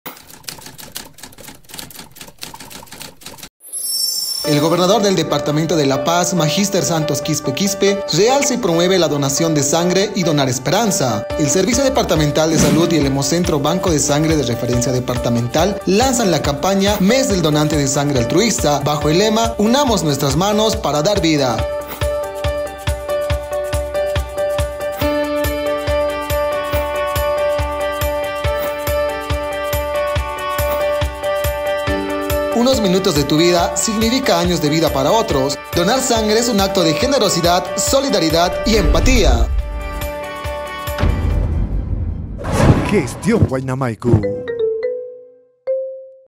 LANZAMIENTO MES DEL DONANTE DE SANGRE ALTRUISTA
MES DEL DONANTE DE SANGRE ALTRUISTA
El Gobernador del Departamento de La Paz, Santos Quispe Quispe, y el Servicio Departamental de Salud SEDES La Paz a traves del HEMOCENTRO-Banco de Sangre de Referencia Departamental de La Paz, lanzan la campaña "Mes del Donante de Sangre Altruista" con el lema "UNAMOS NUESTRAS MANOS PARA DAR VIDA".